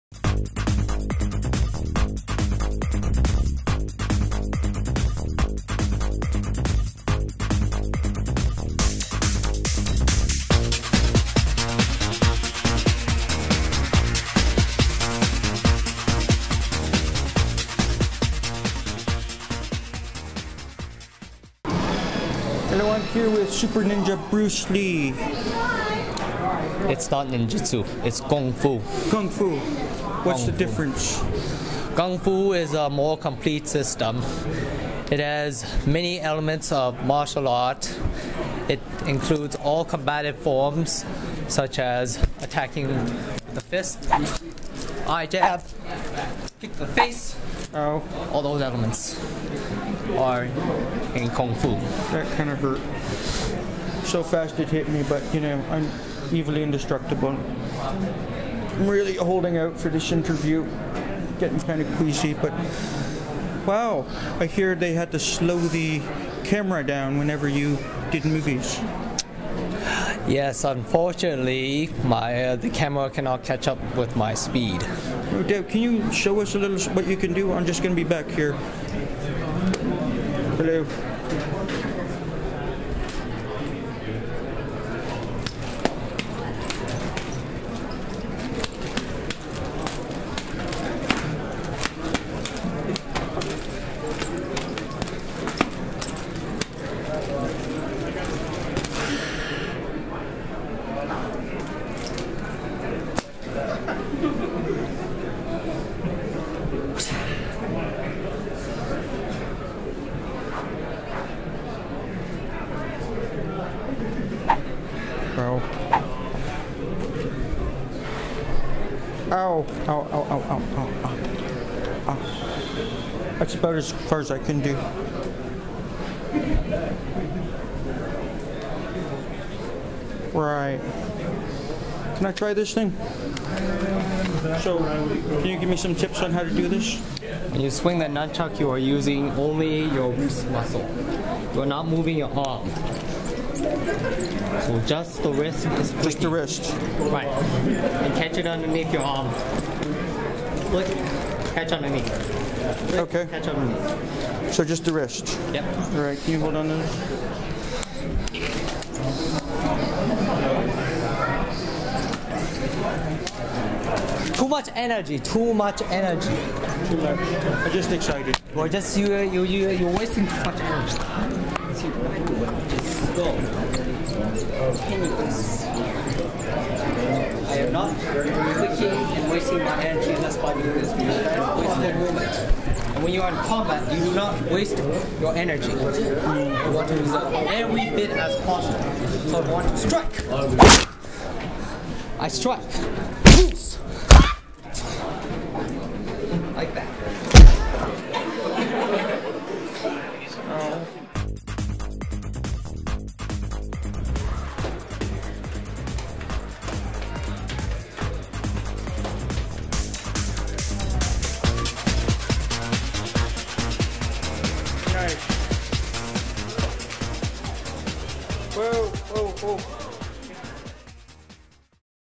Bruce Lee Cosplayer
Dr Stevil gets a lesson in Kung Fu from Bruce Lee